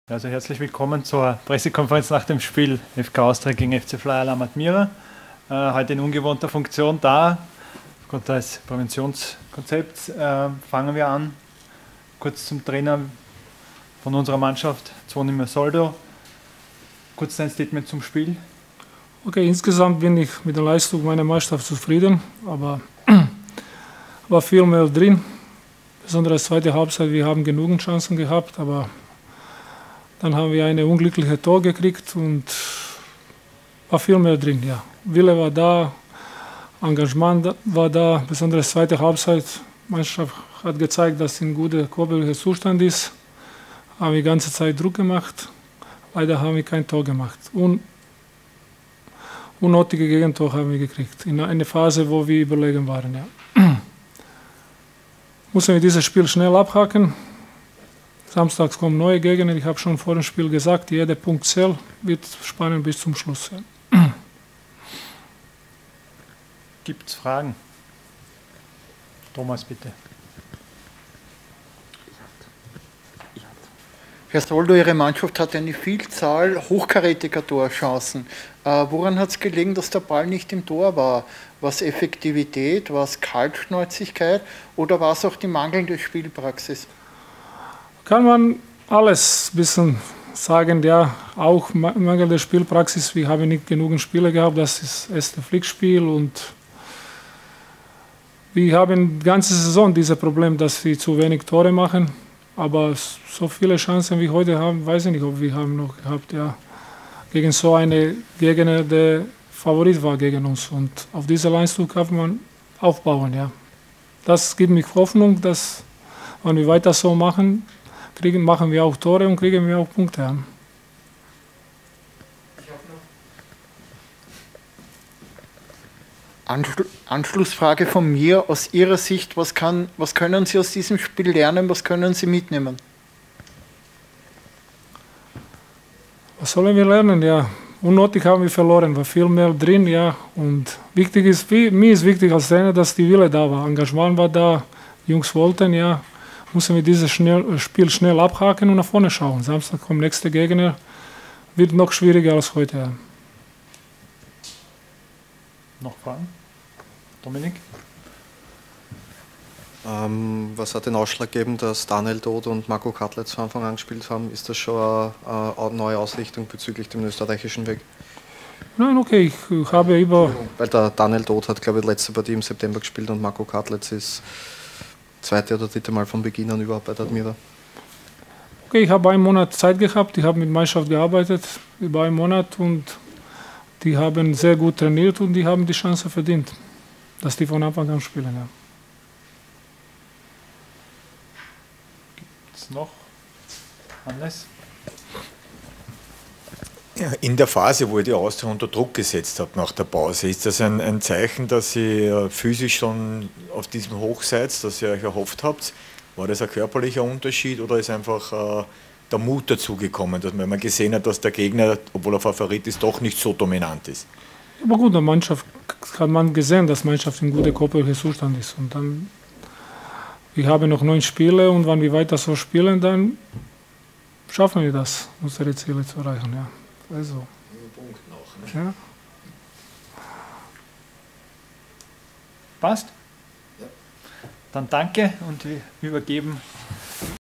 Pressekonferenz mit Admira-Trainer Zvonimir Soldo nach der 0:1-Niederlage bei der Austria
Am Dienstag setzte es für die Admira, in der 23. Runde der Tipico Bundesliga, eine 0:1-Auswärtsniederlage bei Austria Wien. Nach dem Spiel stand der Trainer der Niederösterreicher Zvonimir Soldo den Journalisten Rede und Antwort. Wir waren für euch – wie gewohnt – mit der Kamera dabei.